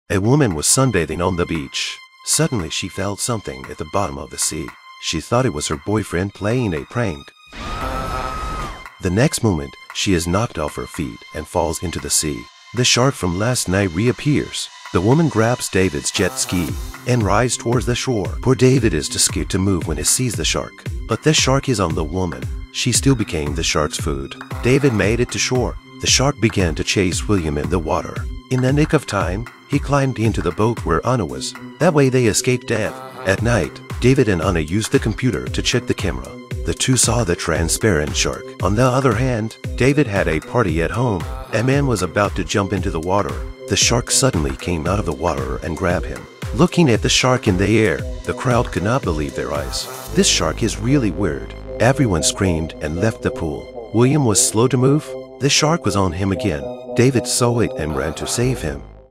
A Shark Dies And Turns Into A Ghost Shark And Flies Into The City To Attack Humans.
The rude and brutal Bobeck (Kim Collins) and his daughter Victoria (Amy Brassette) participate in a fishing competition, but the fish they caught is eaten by a white shark. Bobek is furious and uses dynamite against the shark, which leads to a terrible death for father and daughter, and even the captain of the yacht is not spared. The next day, Ava (Mackenzie Rosman), Cameron (Jaren Mitchell), Mick (Shawn C. Phillips), Bladesey (Dave Randolph-Mayhem Davis) and several other young people came to the beach to play, but in the joy of a fluorescent shark to people launched a A shark with a luminescent body attacked people violently. It turns out that the shark also has a hateful and resentful heart, although the shark last night was killed by Bobek, but its resentment makes it transformed into a vengeful ghost shark, to all humans to launch revenge.
And behind all this, and hide the secret of the unspeakable .